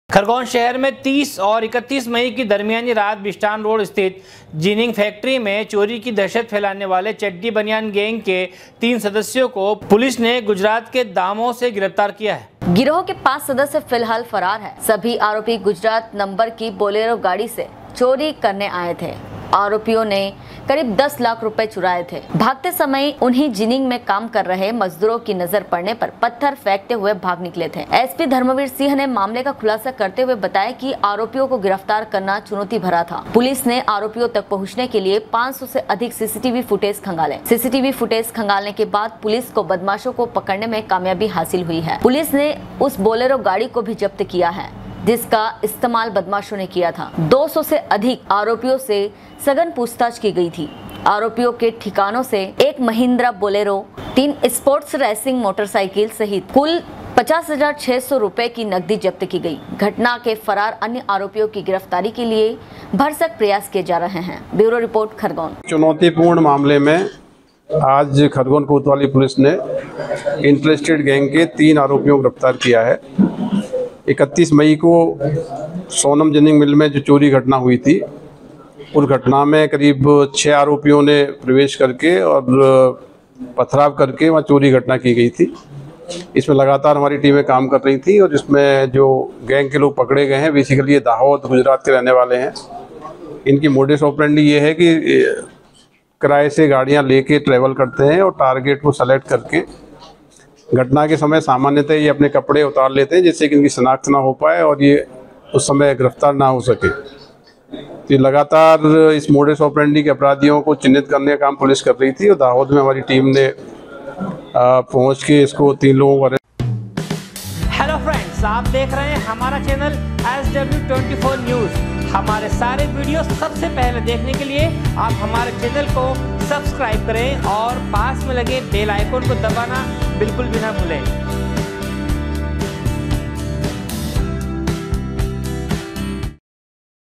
जिनिंग फैक्टरी में चोरी कर दहशत फैलाने वाले चड्डी बनियान गैंग के तीन सदस्यों को गिरफ्तार किया है।
खरगोन शहर में 30-31 मई की दरमियानी रात बिस्टान रोड स्थित जिनिंग फैक्टरी में चोरी कर दहशत फैलाने वाले चड्डी बनियान गैंग के तीन सदस्यों को पुलिस ने गुजरात के दाहोद से गिरफ्तार किया है।